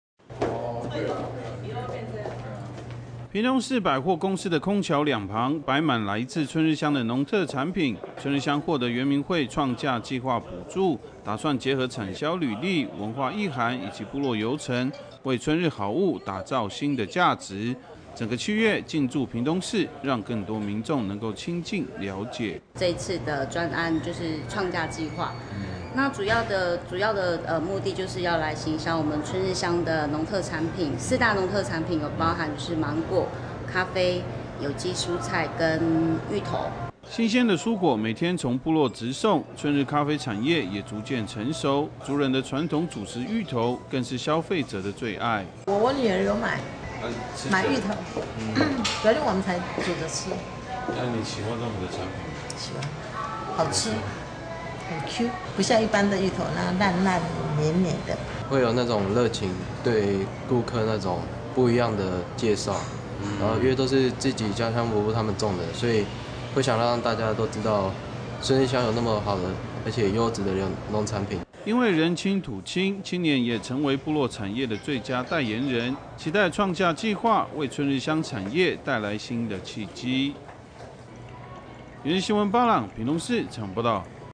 春日四大農產進駐百貨公司 創造產銷新價值 2020-07-10 IPCF-TITV 原文會 原視新聞
屏東市百貨公司的空橋兩旁，擺滿來自春日鄉的農特產品，春日鄉獲得原民會創價計畫補助，打算結合產銷履歷、文化意涵以及部落遊程，為春日好物打造新的價值，整個7月進駐屏東市讓更多民眾能夠親近、了解。

新鮮的蔬果每天從部落直送，春日咖啡產業也逐漸成熟，族人的傳統主食芋頭，更是消費者的最愛。

因為人親土親，青年也成為部落產業的最佳代言人，期待創價計畫為春日鄉產業帶來新的契機。